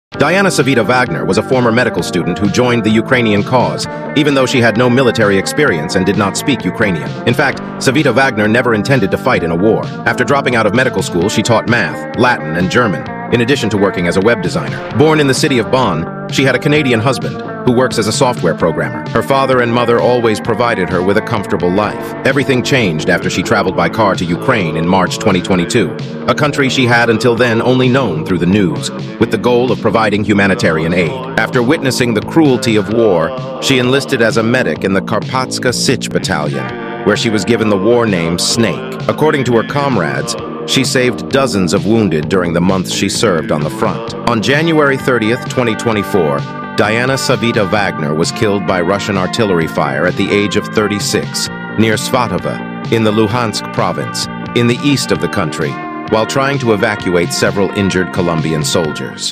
THE GERMAN MEDICAL STUDENT WHO FOUGHT IN UKRAINE
Diana Savita Wagner was a former medical student who joined the Ukrainian cause, even though she had no military experience and did not speak Ukrainian.

1️⃣@alexandersa1166 US$ 30,00
2️⃣@rtbubs77 US$ 9,99

🟢CHANNEL MEMBERS: @clintsiddell9050 @williamstevens6615 @drevakelemen52n92 @jeremypowell104 @markshields9764 @isaacsantana2884